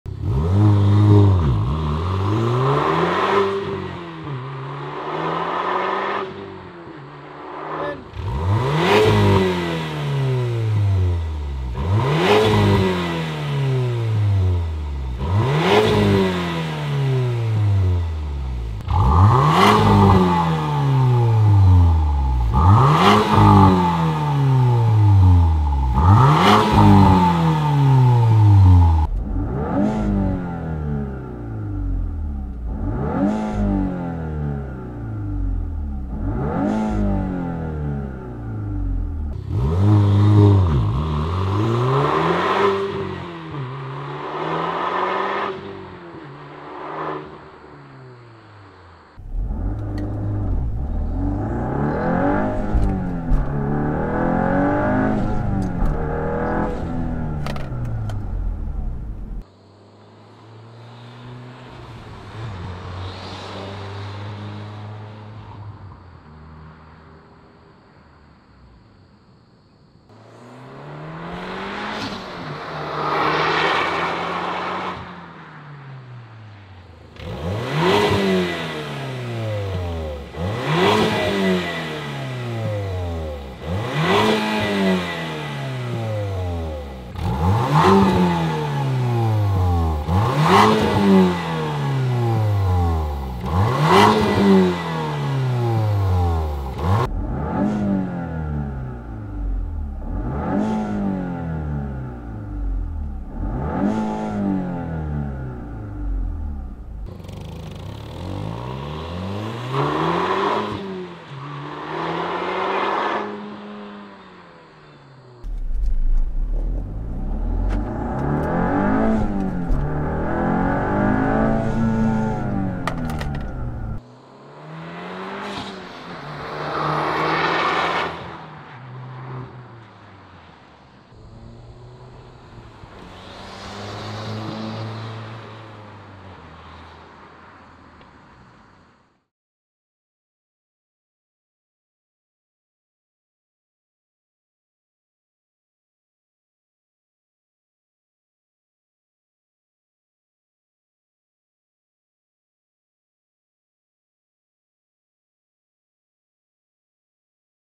MK8 Golf R Catback Exhaust | ECS Product Highlight
Are you ready to radically transform the driving experience in your Volkswagen MK8 Golf R? Take your docile, stock exhaust and replace it with the ECS Tuning in-house engineered Valved Catback Exhaust System! Our proprietary valved muffler design allows you to toggle between a mild-mannered, sporty Euro. exhaust note, with the valves closed, to a deep, aggressive sound with the valves open. Intoxicating exhaust notes fuel your desire to push your engine to its limits with every ounce of power and every engine revolution. Experience true driving passion with our ECS Valved Catback Exhaust System!